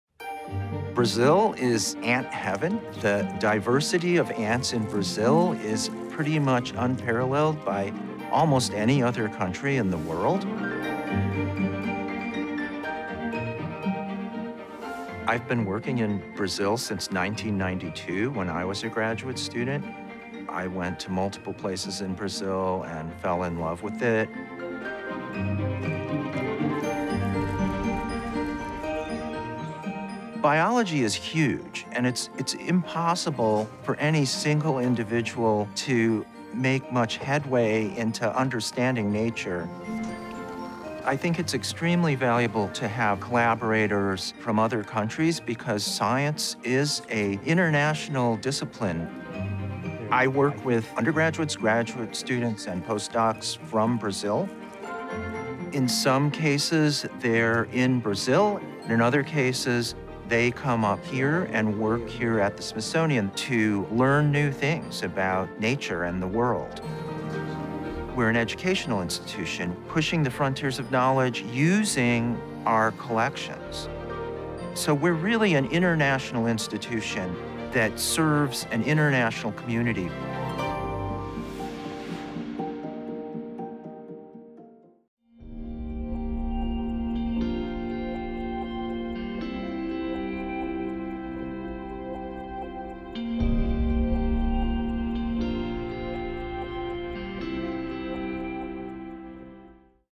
Meet the Curator: Ted Schultz
Ted Schultz , Research Entomologist and Curator of Hymenoptera at the Smithsonian National Museum of Natural History, tells us about his love for ants and also explains why science transcends countries and borders.

The Smithsonian is here for you from home with vast digital resources to educate, inspire, and support.